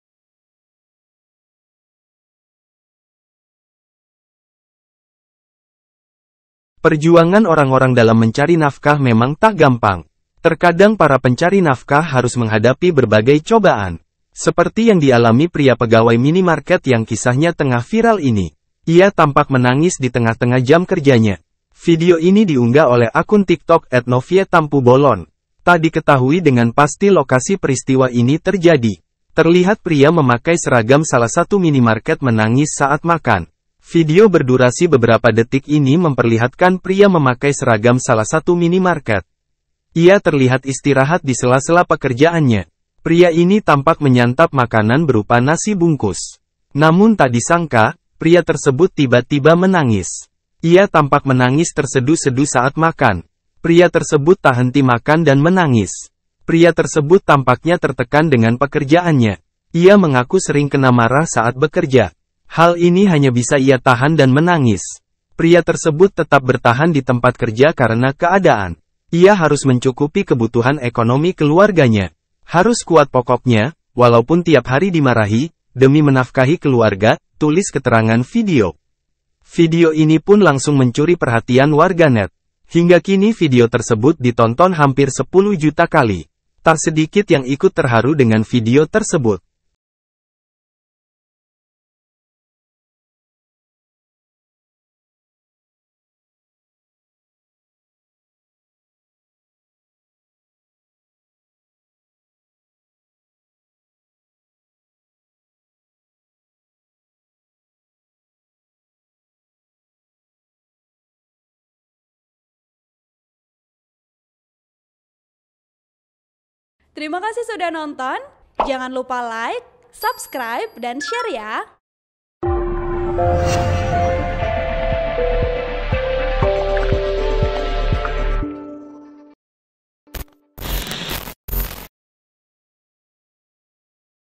Viral Video Pria Pegawai Minimarket Menangis saat Makan, Bikin Haru
Perjuangan orang-orang dalam mencari nafkah memang tak gampang. Terkadang para pencari nafkah harus menghadapi berbagai cobaan. Seperti yang dialami pria pegawai minimarket yang kisahnya tengah viral ini. Ia tampak menangis di tengah-tengah jam kerjanya.

Editor Rheymeldi
Sumber Tiktok/ novietampubolon